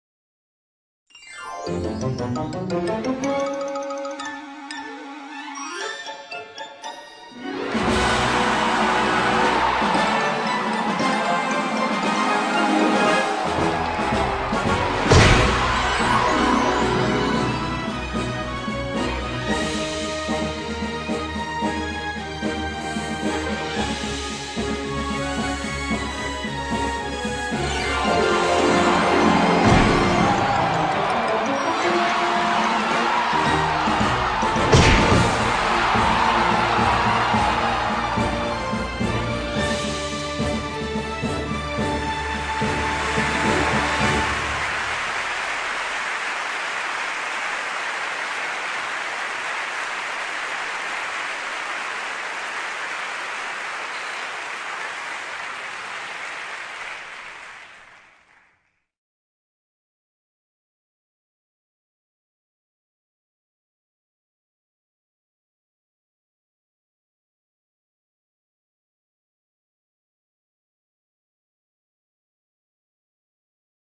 Mondiali 2010 - La mascotte.flv
il cartone animato di presentazione dei Campionati Mondiali di volley maschile Italia 2010